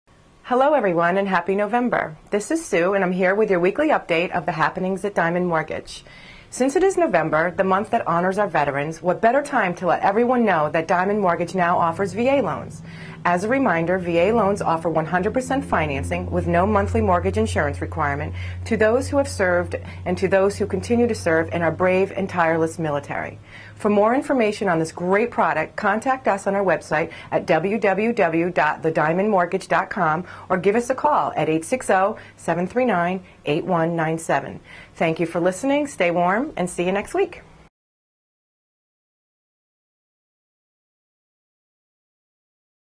Diamond Mortgage Group Weekly Update #1 - VA loans now available!
In this video, Susan E. Shepherd discusses the VA loans now available from The Diamond Mortgage Group.

The Diamond Mortgage Group TM specializes in helping guide you throughout the mortgage or refinancing process.